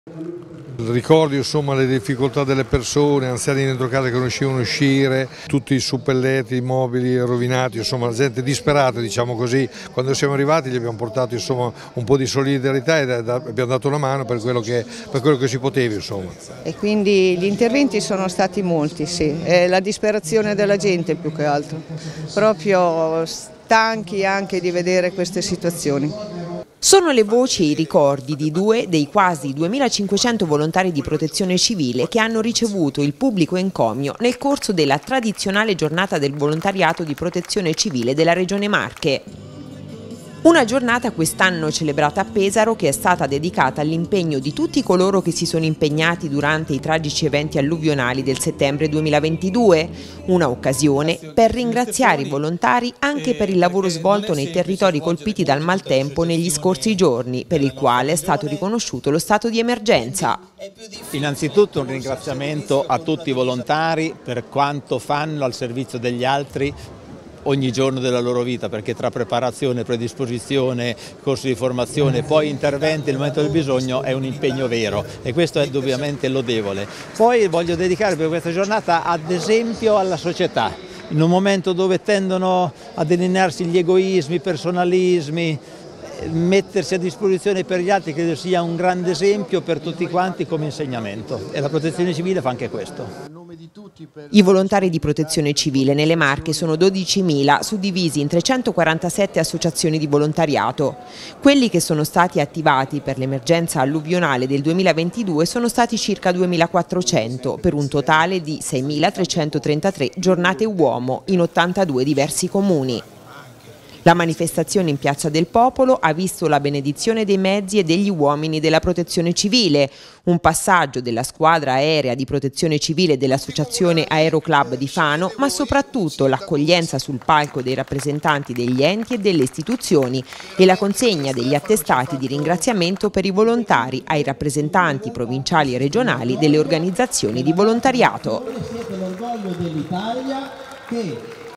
Giornata Volontariato Protezione Civile: riconoscimento per l'impegno durante l'alluvione del 2022
Aguzzi: “Siete l’esempio di chi si mette a disposizione della società”